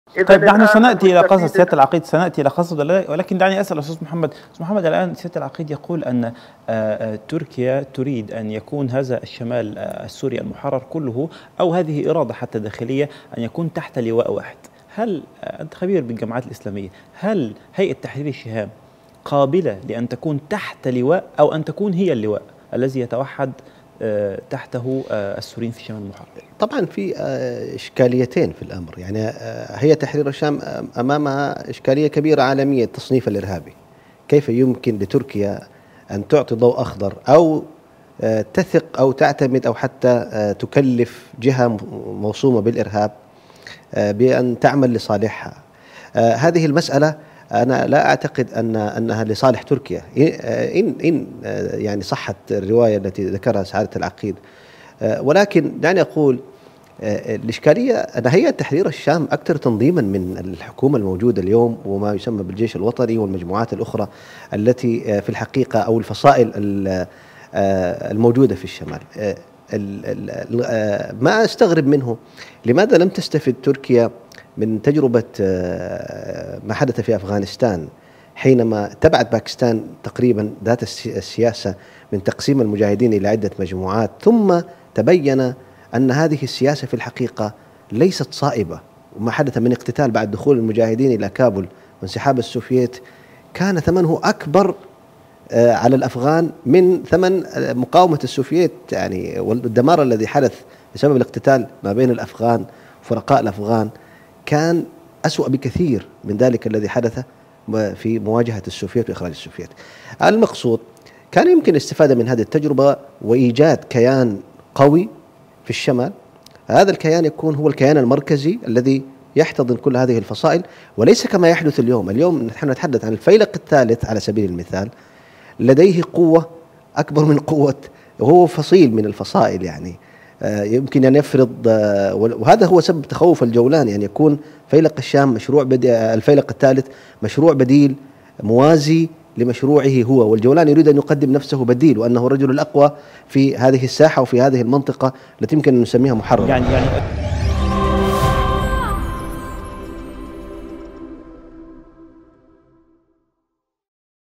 من سيقود الشمال السوري المحرر بعد عملية عفرين .. هل تصبح القيادة بيد هيئة تحرير الشام ؟
Nilesat (النايل سات): 11603 أفقي